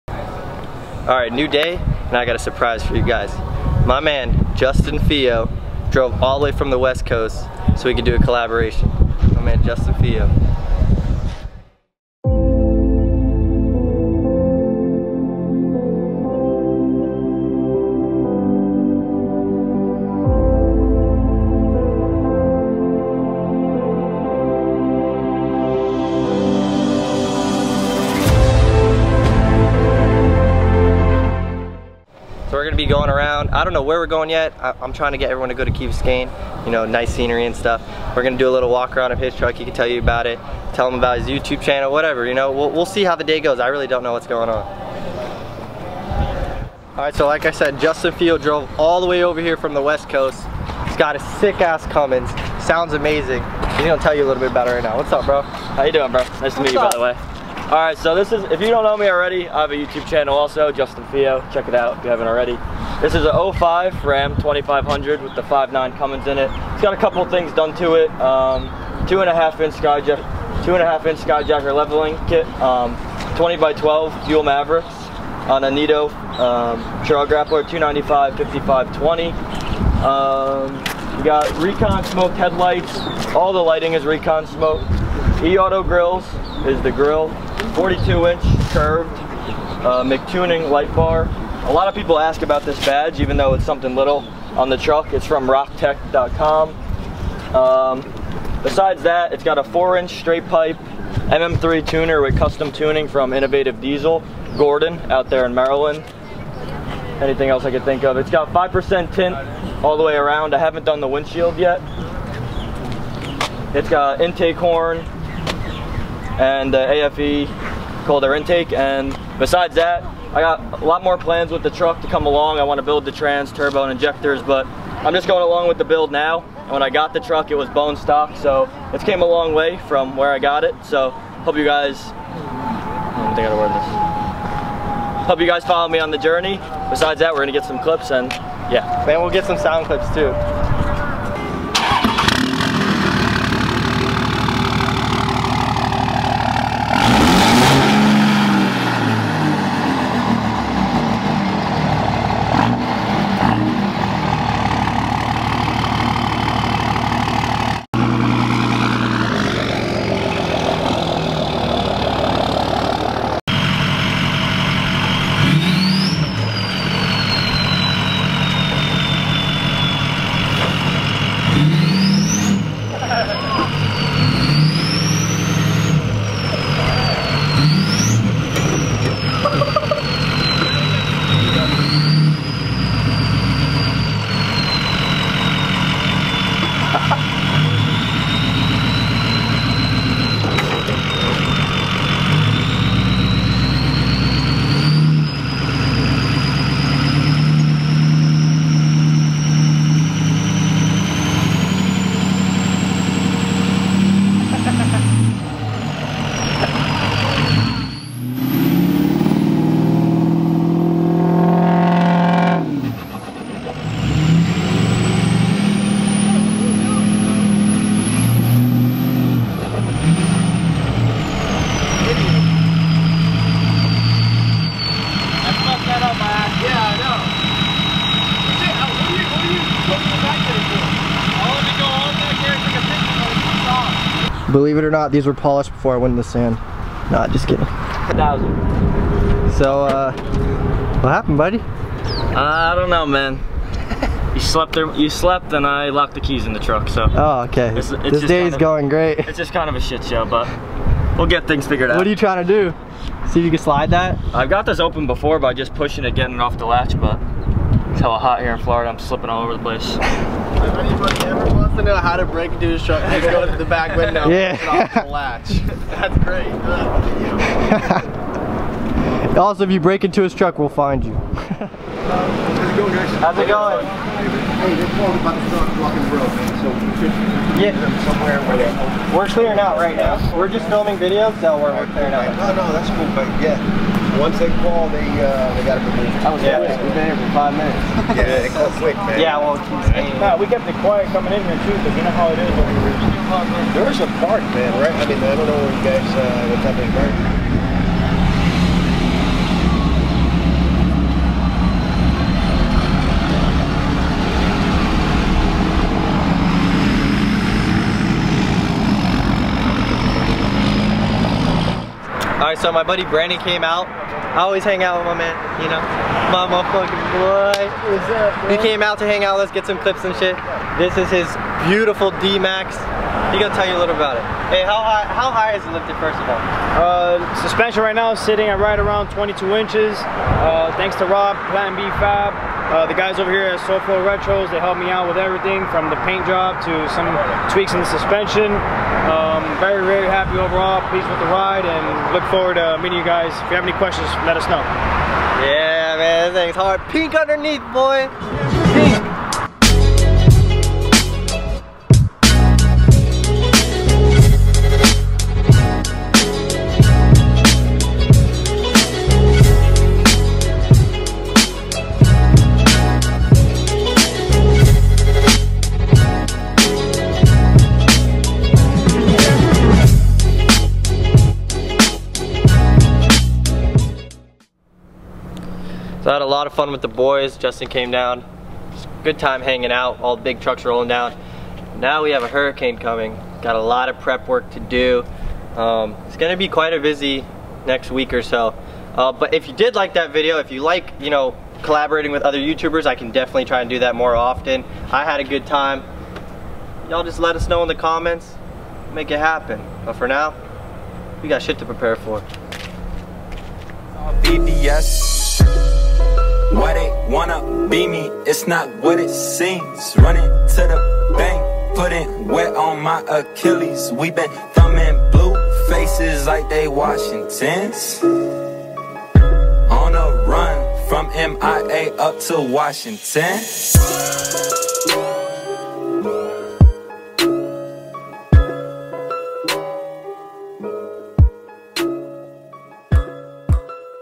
MASSIVE Lifted Trucks Take Over Miami
Mobbing around Miami with Justin Fio! If you enjoyed this collaboration comment and tell me to do more!